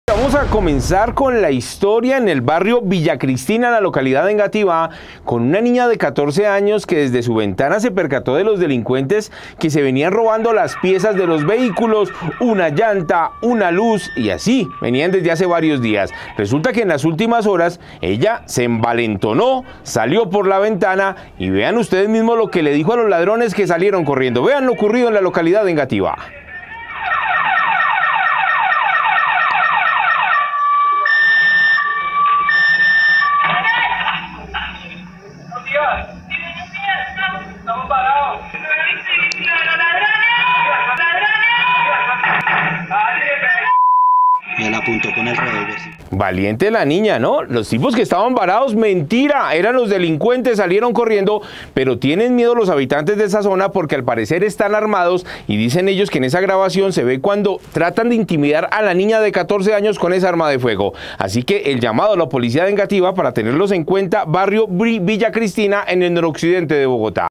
Niña se enfrentó a ladrones armados que estaban robando autopartes en Bogotá
Salió por la ventana y después de escuchar a los delincuentes decir que supuestamente estaban varados, ella empezó a gritar y los obligó a huir. En medio de la fuga, lo sujetos dispararon.